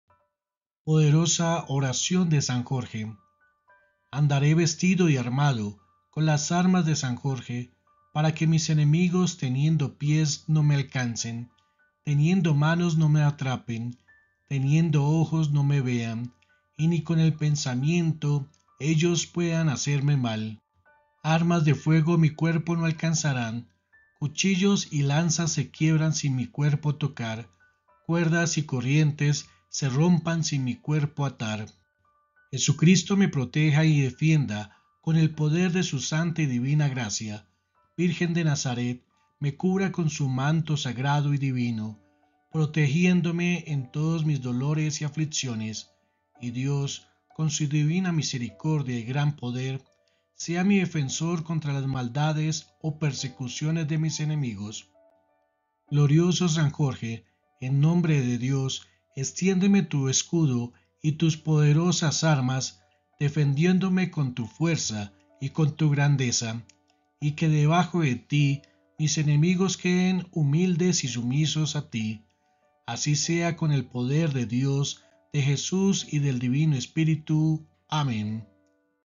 Poderosa Oración de San Jorge: Vence Tus Batallas con Fe
Bienvenidos a nuestro canal de espiritualidad y devoción. En este video, te presentamos la poderosa oración de San Jorge, el valiente patrón de los guerreros. San Jorge es conocido por su capacidad para vencer las batallas y proteger a quienes lo invocan con fe.

Esta oración es un faro de esperanza en tiempos de dificultades. Aprende cómo la devoción a San Jorge puede brindarte fortaleza y ​​protección en tus desafíos diarios.

La fe en San Jorge puede ayudarte a vencer tus propias batallas. ¡Gracias por unirte a nosotros en esta devoción!